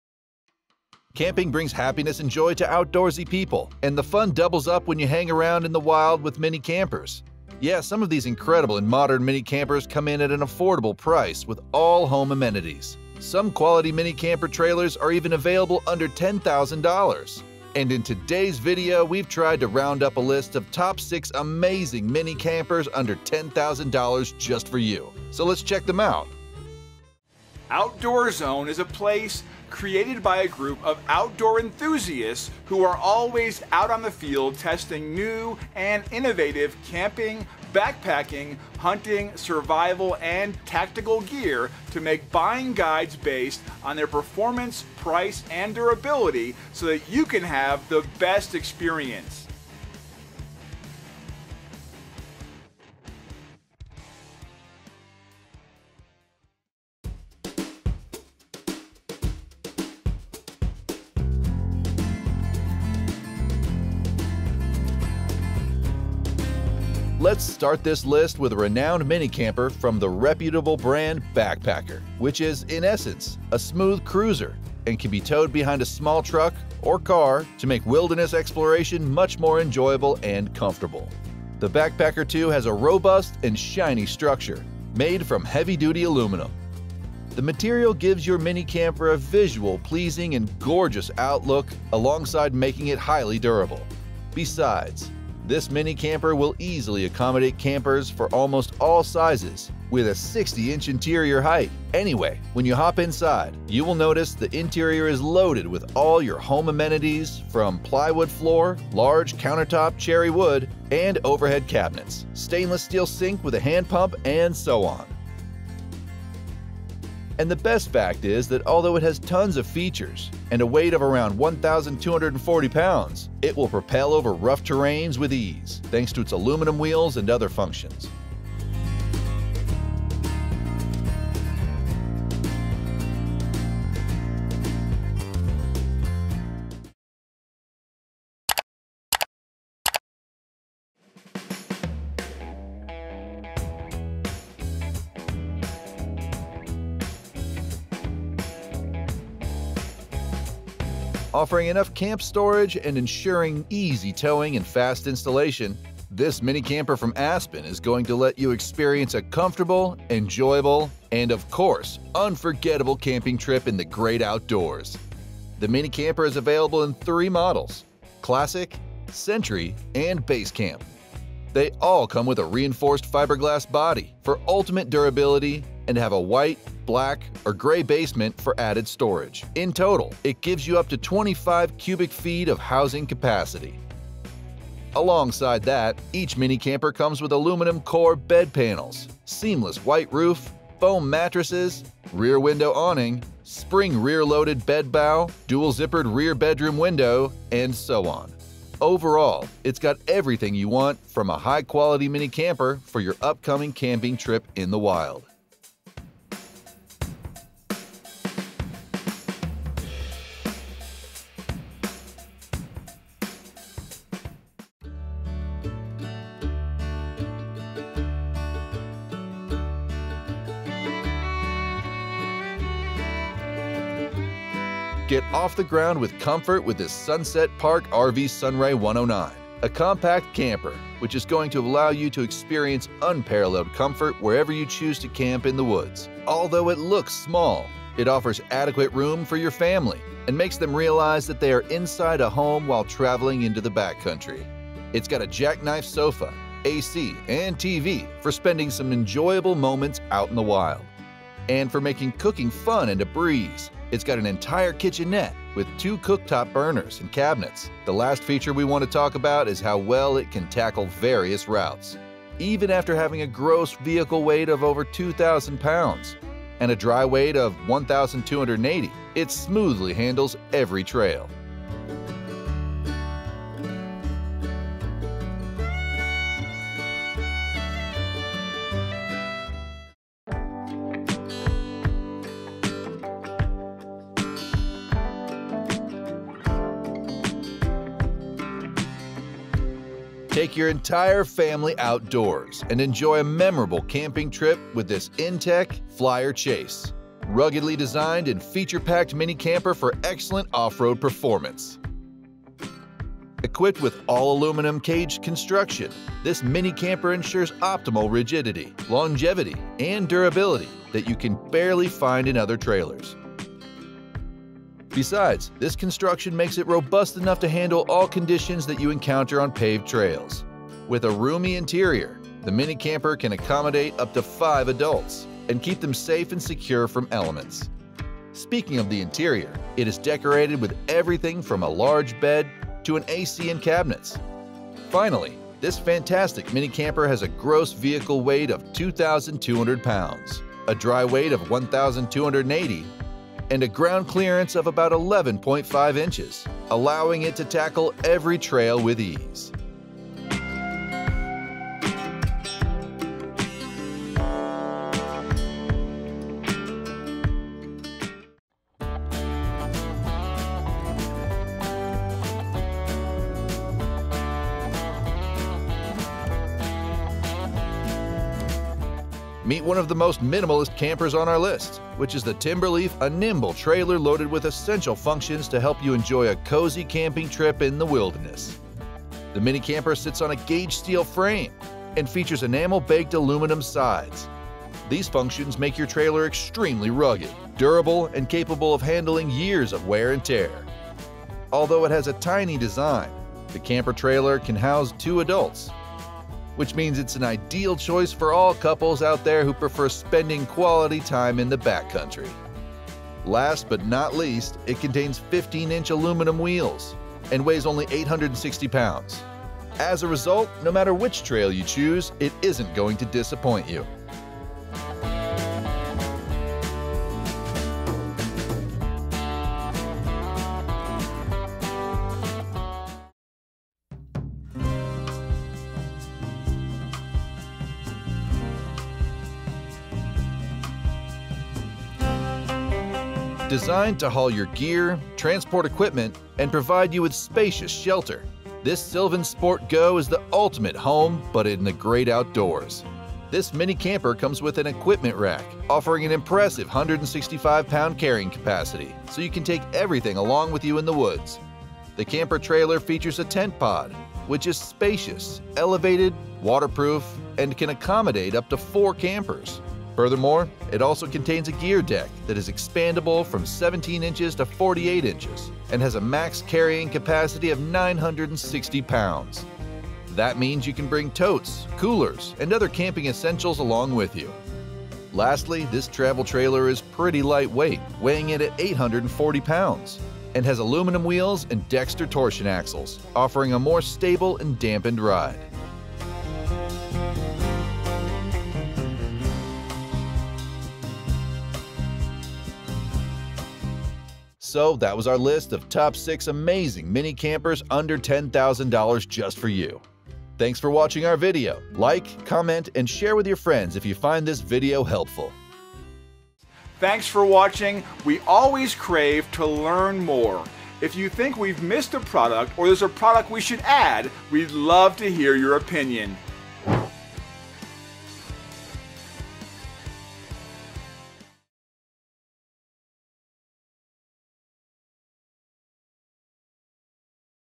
Top 6 Innovative Mini Campers Under $10,000
There are some innovative mini campers, which can give you, your family, or a group of campers a secure feeling and home-like amenities in the great outdoors. And you can find some affordable options too. In this video, we've talked about the top 6 innovative mini campers under $10,000!

Innovative Mini Campers List:

0:00 - Introduction

00:56 - 6. Backpacker 2

02:20 - 5. Aspen Classic

03:34 - 4. Sunset Park RV Sunray 109

04:39 - 3. inTech Flyer Chase

05:58 - 2. Timberleaf All-Road

07:03 - 1. SylvanSport GO